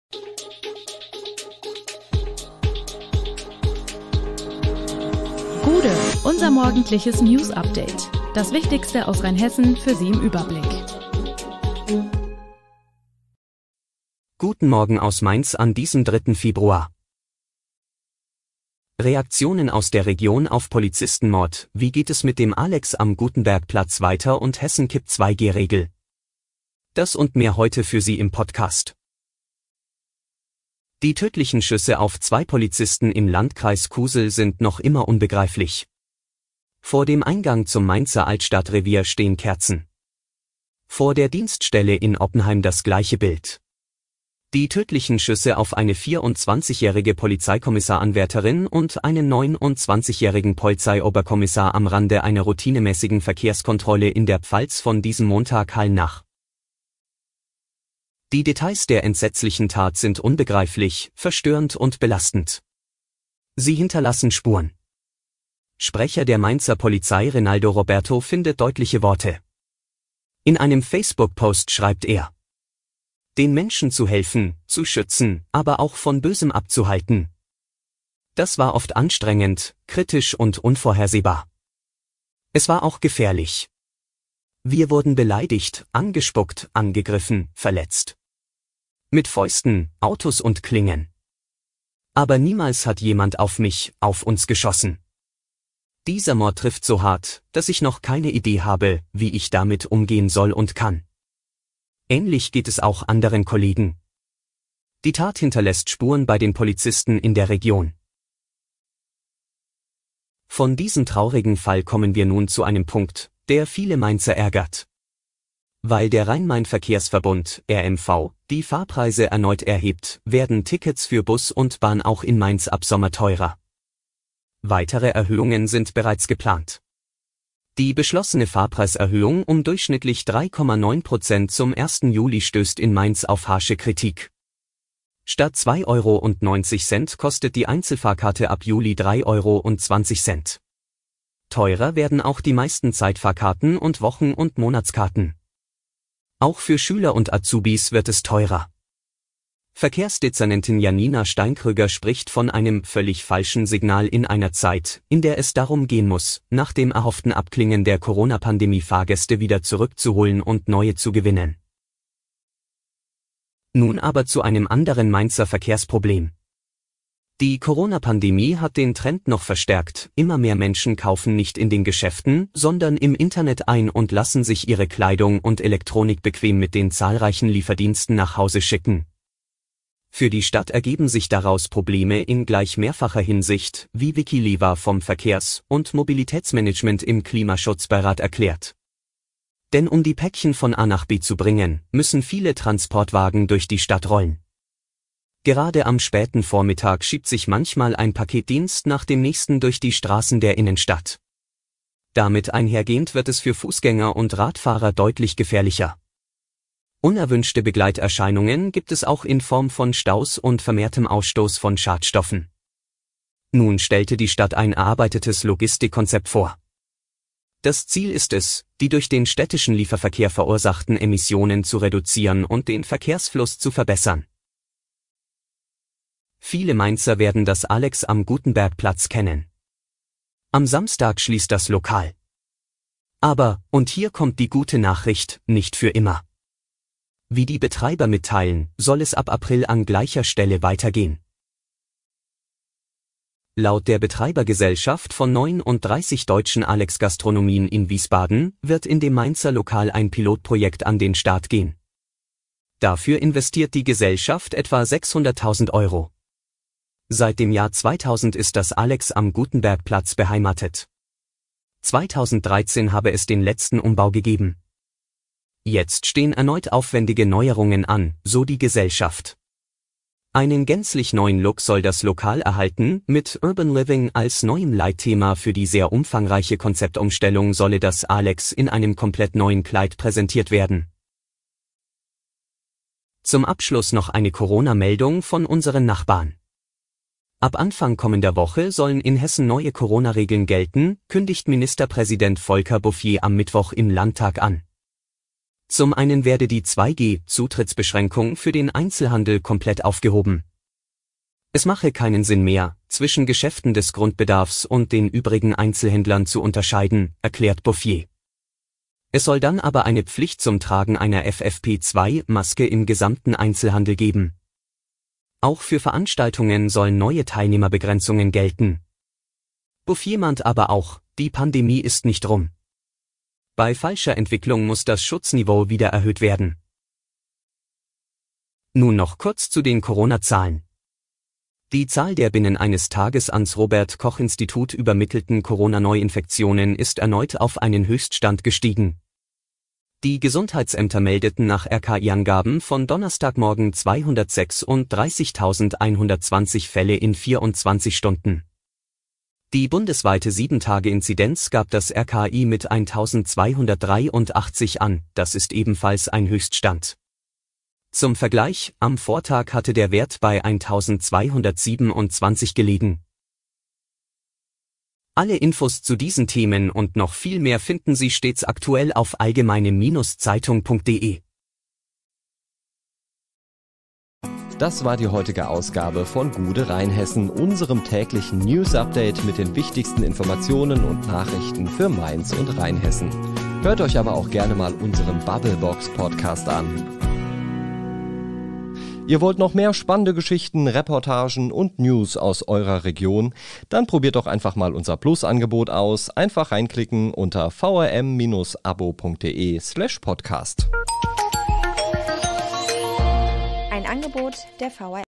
Polizisten aus der Region: „Dieser Mord trifft so hart“ - Gude, Rheinhessen!
Reaktionen aus der Region auf Polizistenmord, Wie geht es mit dem Alex am Gutenbergplatz weiter, Hessen kippt 2 G Regel. Alle Hintergründe zu den Nachrichten des Tages finden Sie hier: